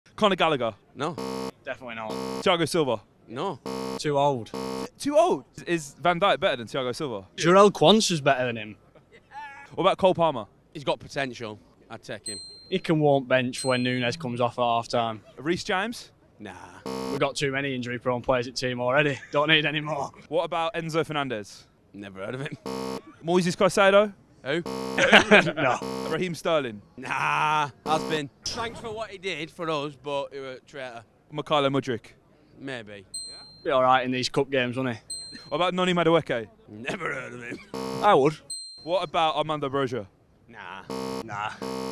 They wouldn't take WHICH CHELSEA PLAYERS?! 😱
Liverpool soccer fans say which players they would take from their Premier League rivals Chelsea before the Carabao Cup final at Wembley Stadium!